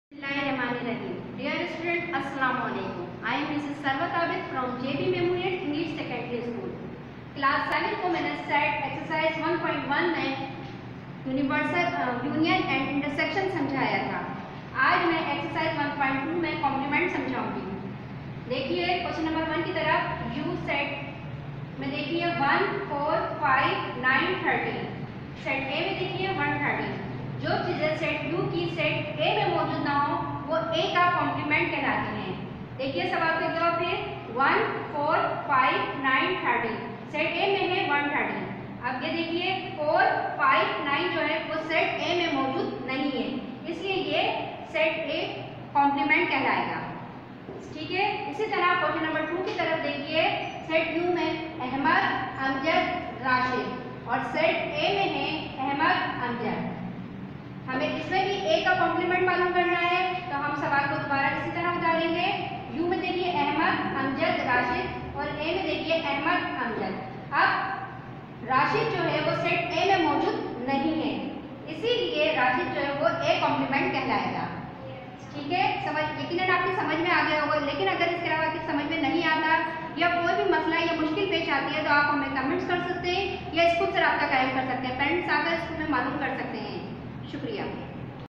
Class VII - MATHEMATICS
Month Of July
Ex: 1.2 (SETS)
Class VII - MATHS

Presented By Miss Sarwat
(Maths Teacher Secondary Classes - JBS)